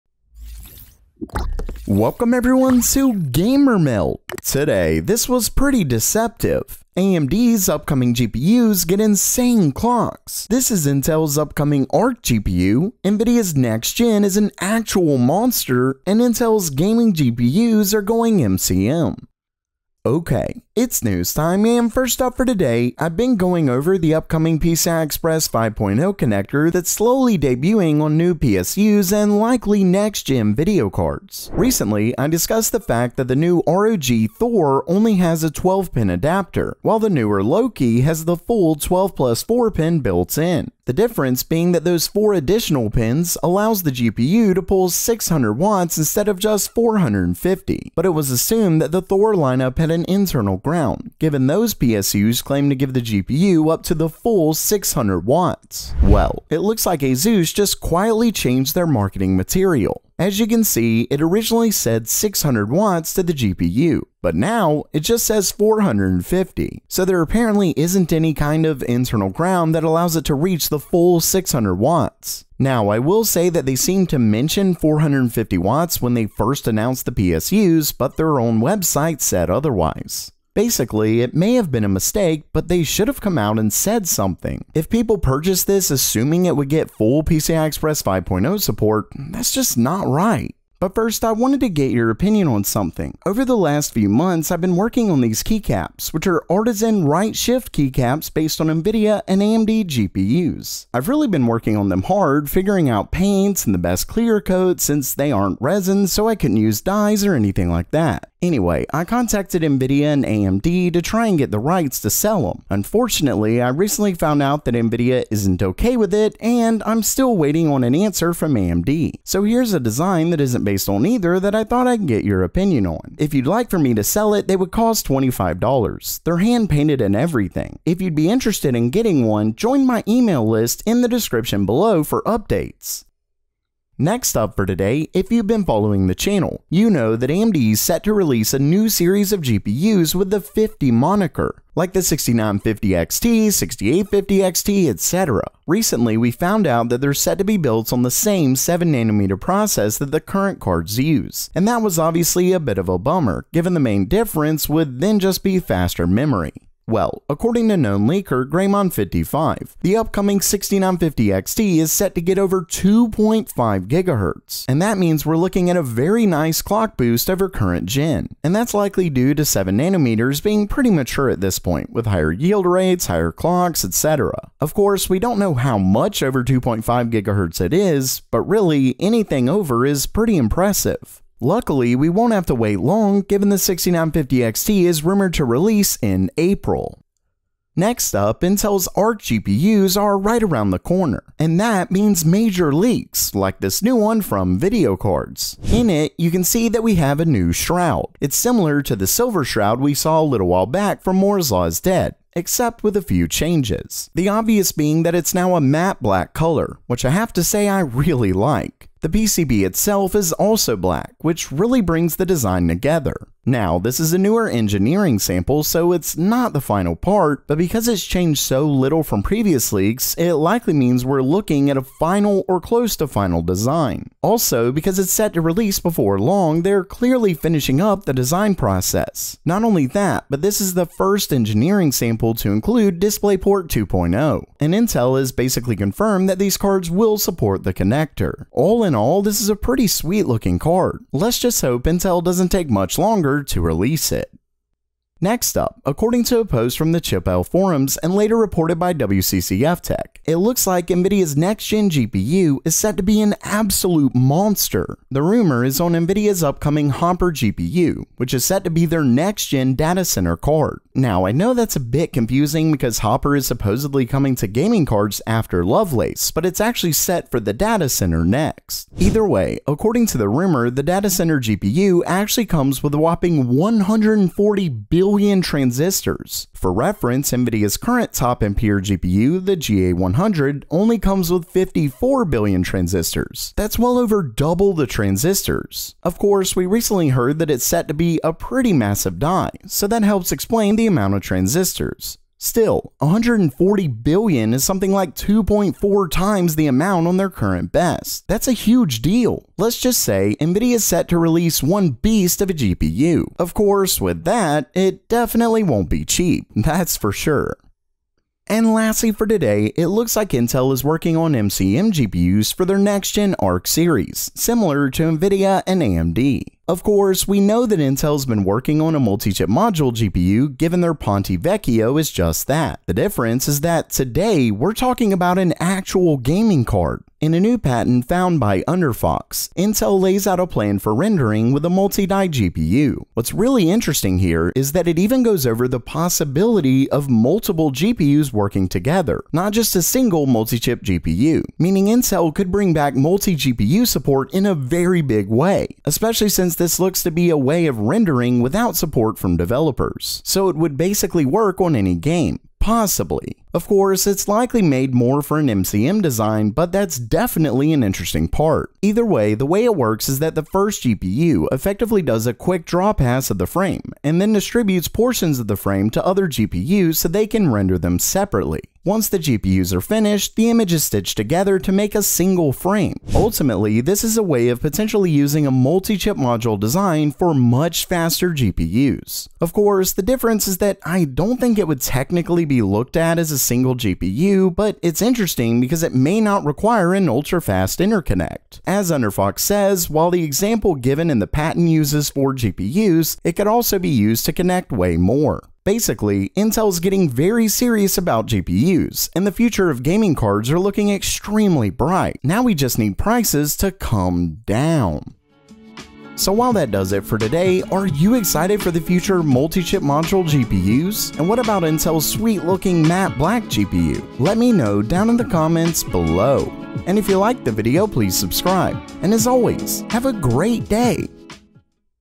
Intel’s ARC GPUs Are Set To RULE THE WORLD!
Intel's ARC GPUs are brining back multi GPU support, ARC Alchemist looks amazing, Nvidia's next Gen is a monster, AMD's upcoming GPUs get insane clocks and this was pretty deceptive!

TIMESTAMPS
0:00 Intro
0:19 This Was Deceptive
2:13 AMD's Upcoming GPUs Get Insane Clocks
3:07 Intel's ARC GPUs Look Amazing
4:05 Nvidia's Next Gen Is A Monster
5:08 Intel's Gaming GPUs Are Going MCM/Multi GPU